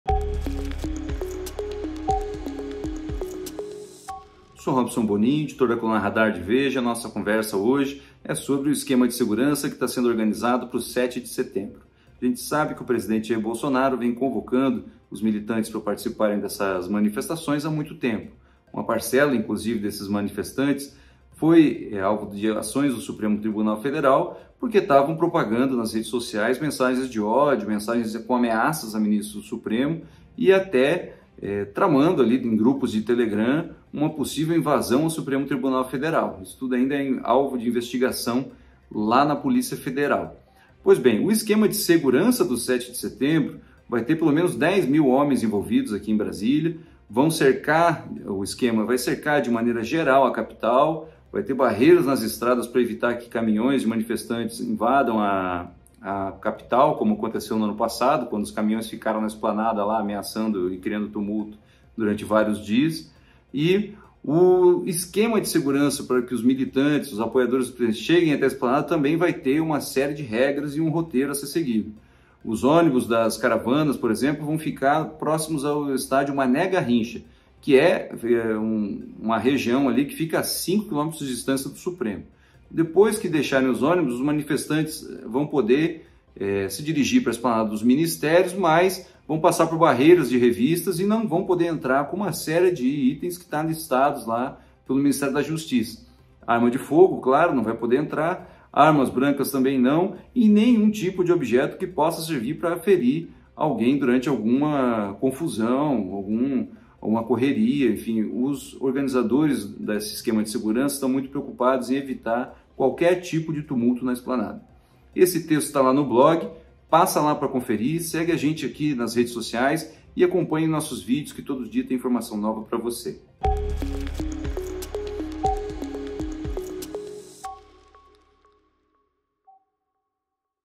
Radar | O pesado esquema de segurança para o 7 de setembro em Brasília
No vídeo, o editor do Radar, Robson Bonin, fala dos preparativos das forças policiais para o feriado do 7 de setembro em Brasília.

Leia mais notas exclusivas sobre política, negócios e entretenimento na coluna Radar